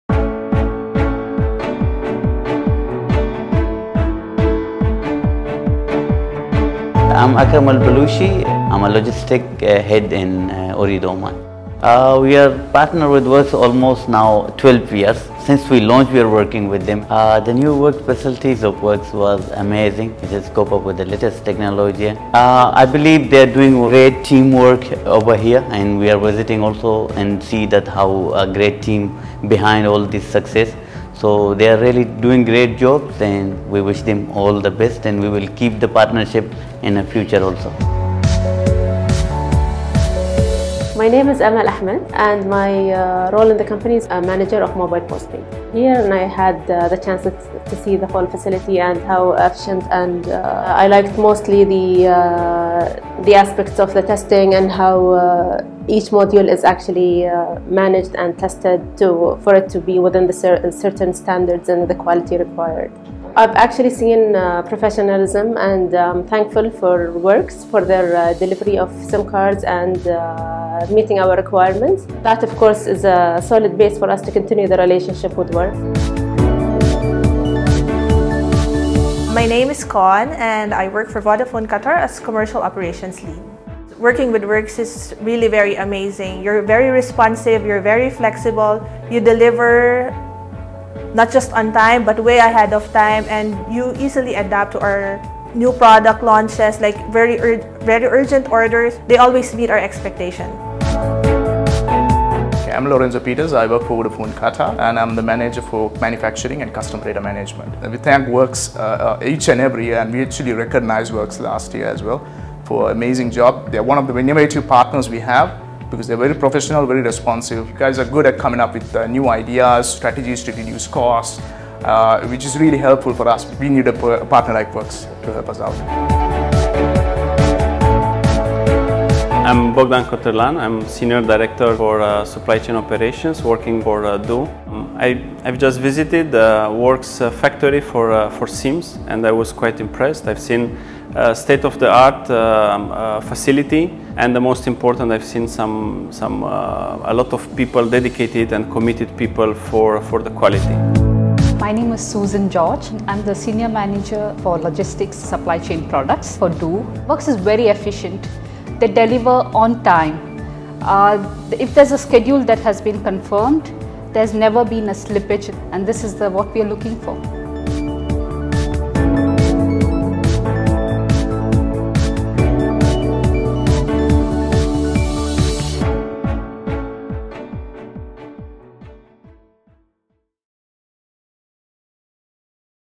Leading telcos visit Workz' Dubai facilities
This video showcases some of the largest telecom operators sharing their experience at the Workz Group's Dubai SIM and scratch card manufacturing facility.

Operators that participated in the factory tour were:

Akram Al Balushi, Head of Logistics, Ooredoo Oman
Amal Ahmed, Manager Mobile Postpaid, Batelco
Con Festin, Commercial Operations Lead, Vodafone Qatar
Lorenzo Pietersz, Manager Manufacturing & CDM, Vodafone Qatar
Bogdan Cotirlan, Senior Director of Supply Chain Operations, Du
Susan George, Senior Manager for Supply Chain Products, Du

Text in the video:

I'm Akram Al Balushi, Head of Logistics at Ooredoo Oman.  We've been partnering with Workz for 12 years, since our launch we've been working with them.  The new Workz facility is amazing, it has the latest technology, and I believe they have a great team that is behind all the success.  They are really doing a great job, we wish them all the best, and we'll keep the partnership going in the future.

My name is Amal Ahmed, Manager Mobile Postpaid at Batelco.  I had the chance to see the facility and see how efficient it is.  I liked mostly the aspects of the testing and how each module is managed and tested for it to be within certain standards and the quality required.  I've seen professionalism, and I'm thankful to Workz for their timely delivery of SIM cards and meeting our requirements.  That, of course, is the solid base for us to continue our relationship with Workz.

My name is Con, and I work for Vodafone Qatar, as Commercial Operations Lead.  Working with Workz is really amazing. You're very responsive, very flexible, you deliver not just on time, but way ahead of time and you easily adapt to our new product launches and very urgent orders.  They always meet our expectations.

I'm Lorenzo Pietersz, I work for Vodafone Qatar, and I'm the Manager for Manufacturing & Custom Data Management.  We thank Workz each and every year, we recognised Workz last year as well for the amazing job.  They are one of the innovative partners we've because they are very professional very responsive.  You guys are very good at coming up with new ideas, strategies to reduce costs which are really helpful for us. We need a partner like Workz to help us out.

I'm Bogdan Cotirlan, Senior Director of Supply Chain Operations, Du.  I just visited Workz factory for SIMs, and I was quite impressed with the state of the art facility and most important I've seen a lot of people dedicated and committed to the quality.

My name is Susan George I'm the Senior Manager for Logistics Supply Chain Products at Du.  Workz is very efficient, they deliver on time if there is a schedule that has been confirmed there's never been a slippage, and this is what we've been looking for.

Thank you for your visit. We look forward to more success together.